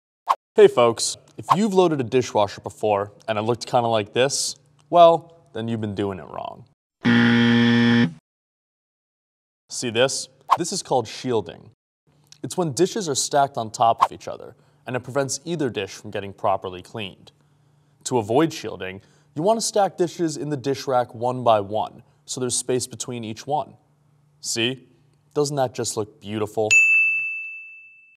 How to Avoid Spotty Dishes
Spotty dishes and wares after a commercial wash cycle are likely due to shielding. What is shielding?

It’s when dishes are stacked on top of eachother, and it prevents either dish from getting properly cleaned.

To avoid shielding, you wanna stack dishes in the disk rack one by one, so there’s space between each one.